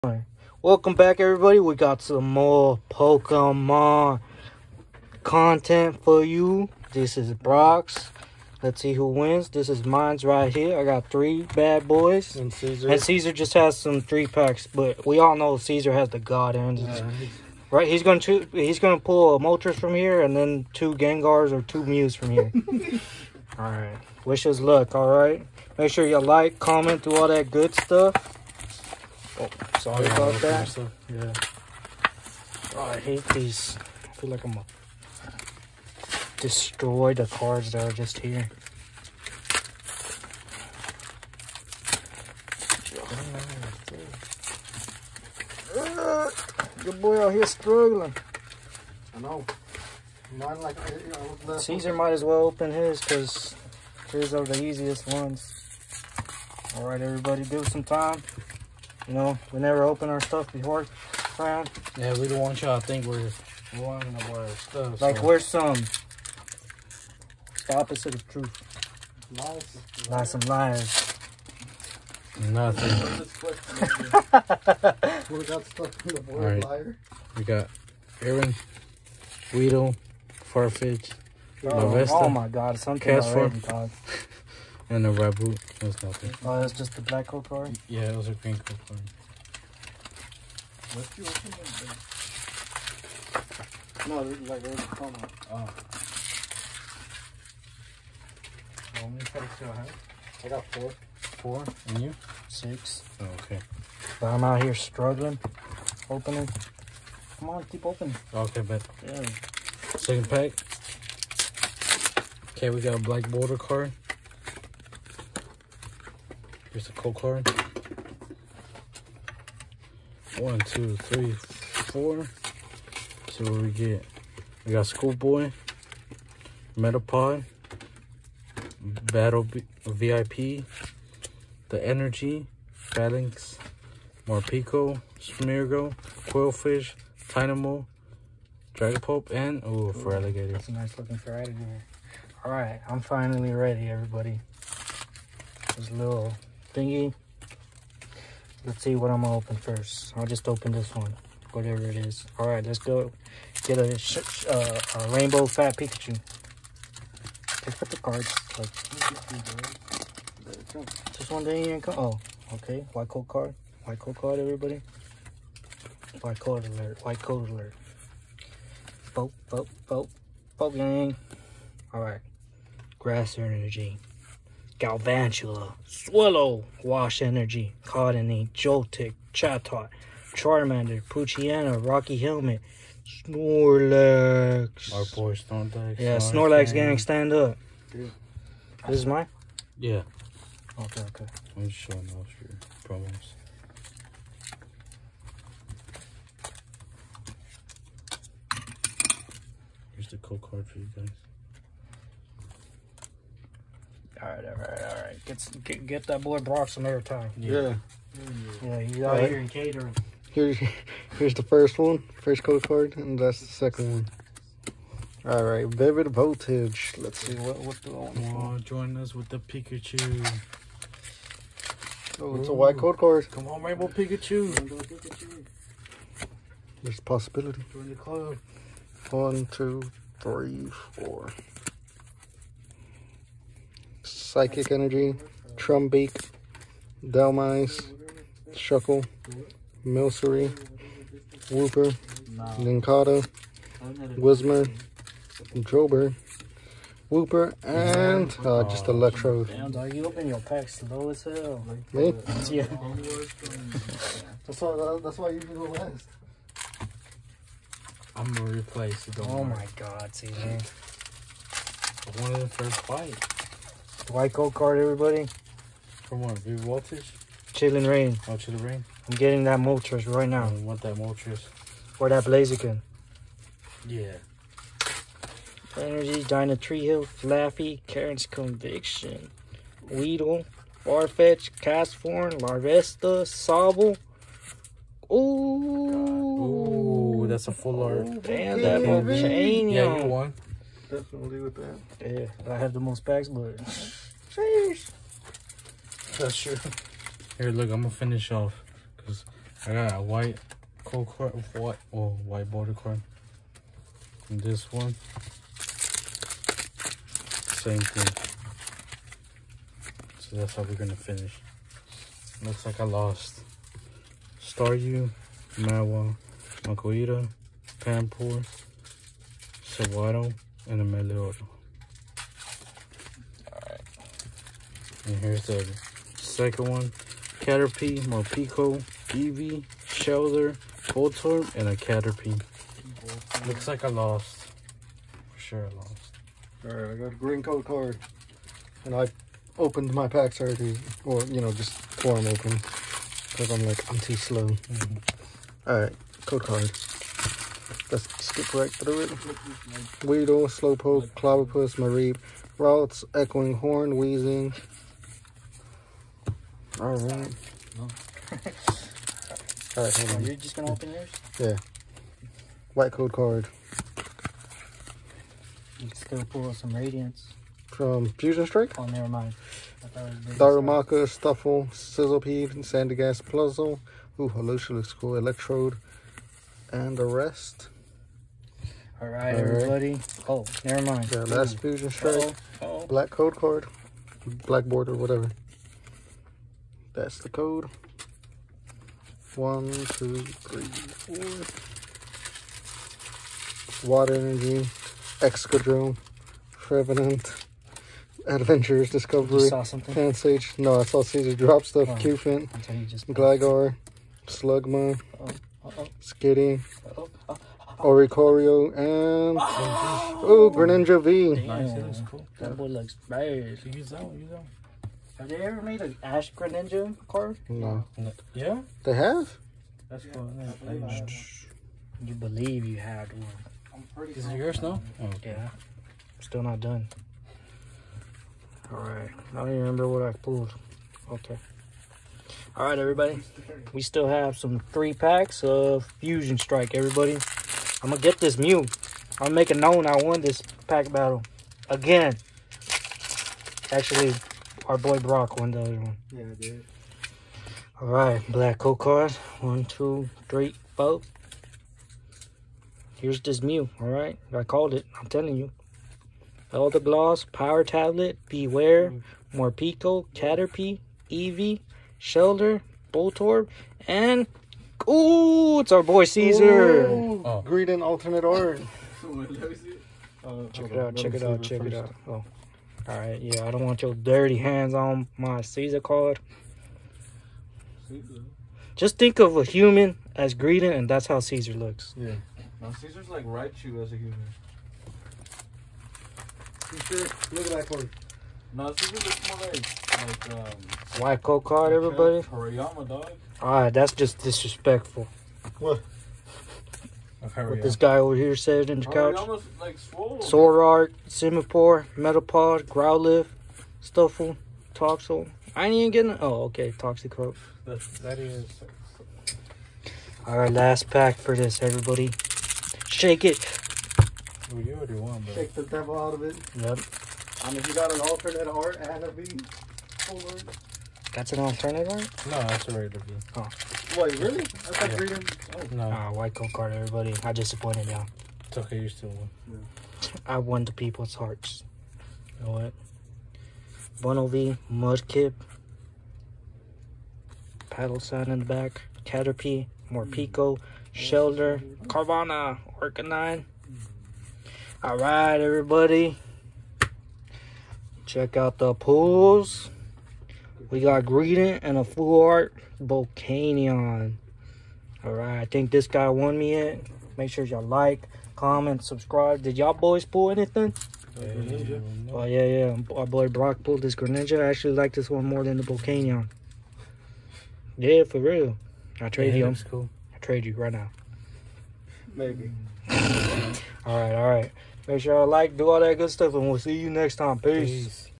Pokémon Coin Battle 2!!! Which is better again?!
Today we battle again with the coin packs of Pokémon also including Cesar’s packs!
Who wins?